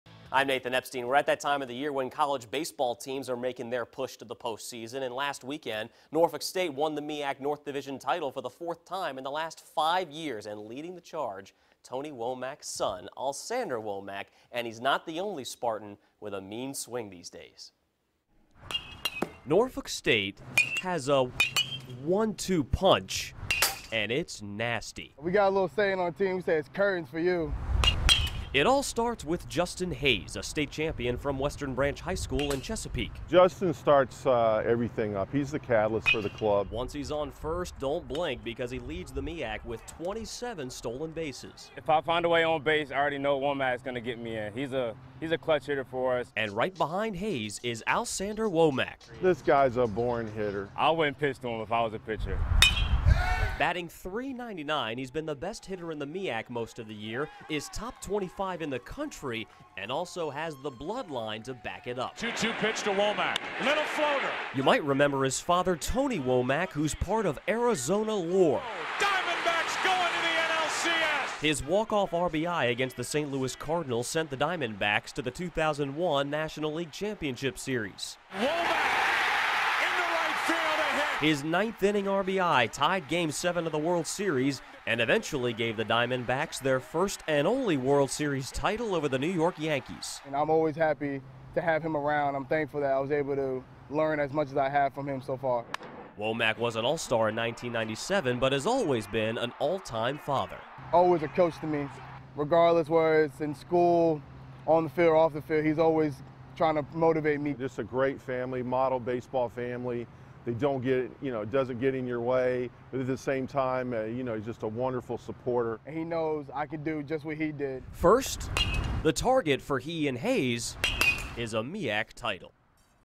Son of a World Series champ, Womack is NSU's top slugger
The apple hasn't fallen far from the tree. Tony Womack was a key player in the Arizona Diamondbacks' run to their first and only World Series title in 2001. His son Alsander has helped Norfolk State win a MEAC North Division championship.